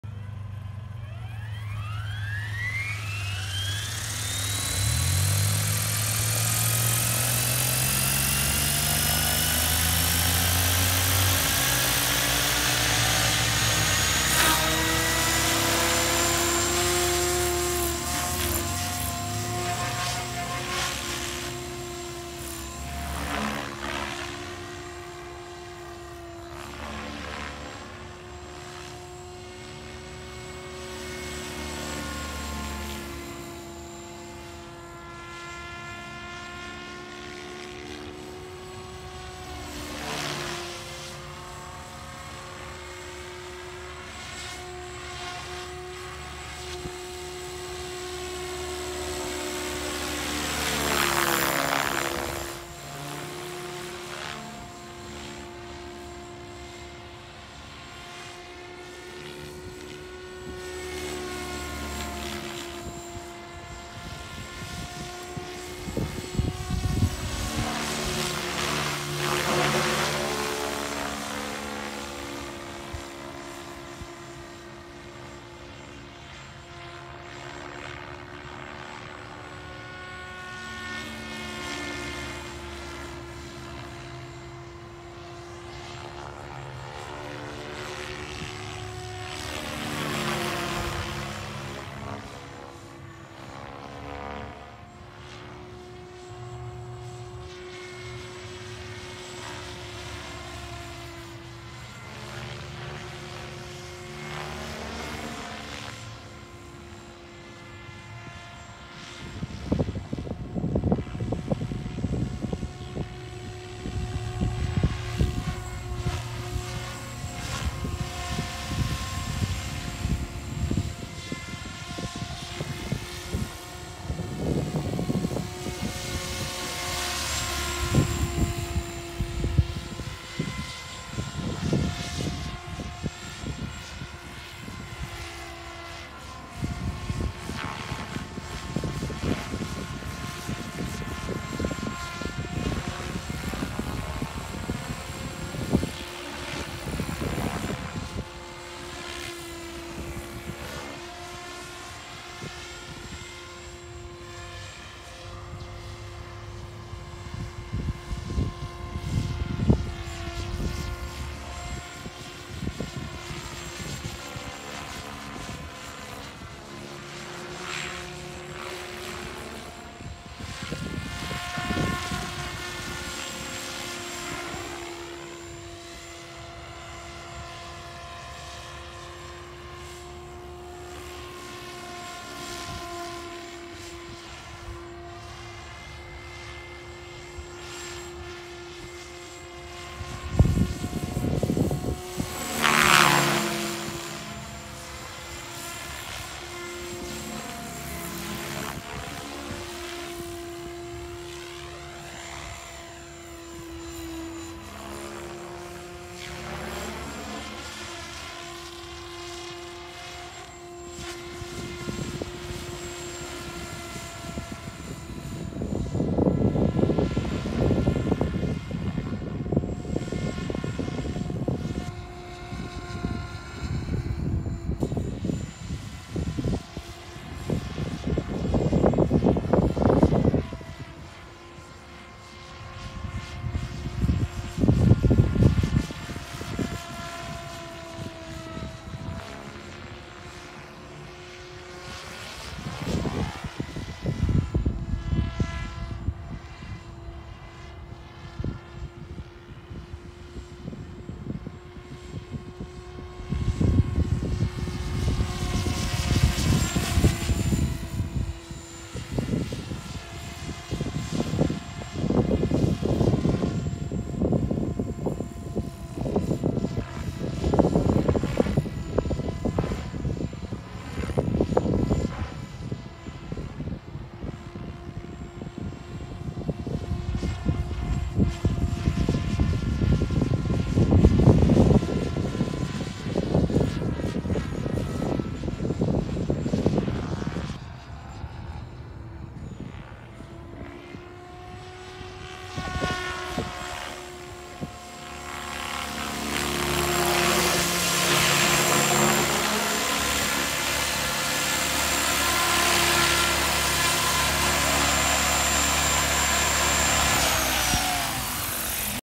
Kraken 580 flight
My new kraken 580 first filmed flight. Needs some tuning tail gain seems too high. Equipment list is:
avian 120amp esc
Xnova 4025 560kv motor
BK cyclic servos
Xpert tail servo
12s setup 2x 6s 3300mah pulse batteries
Spectrum FC6250HX FBL unit
Slxr2 sat with backup sat under chassis